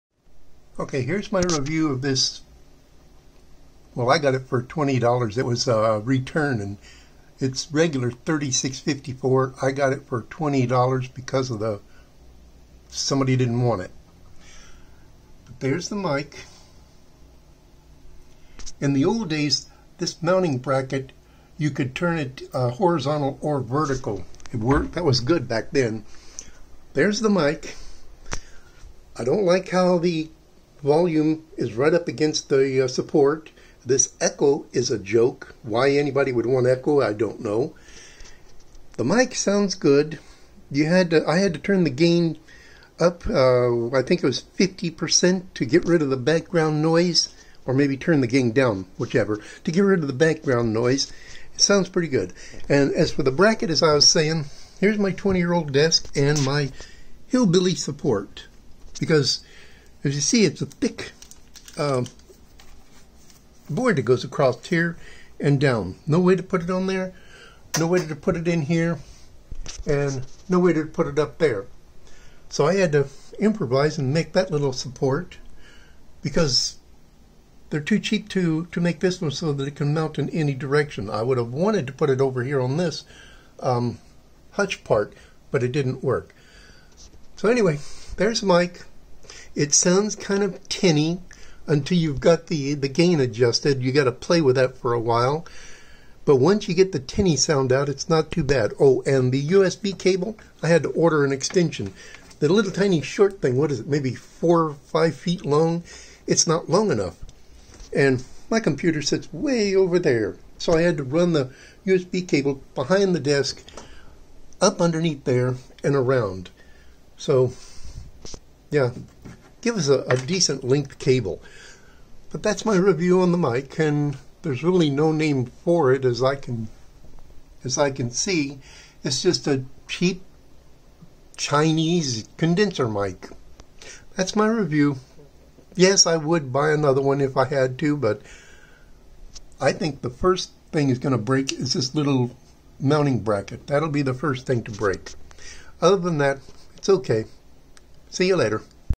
USB Condenser Microphone for Computer review
Got this USB Condenser Microphone for my gaming Computer and thought I would review it for you.

No one complaint is I hear static and a humming sound and my OBS filters can't make it go away, I only hear the sound when in monitor mode and while gaming no on has said anything so I goess it's just in monitor mode.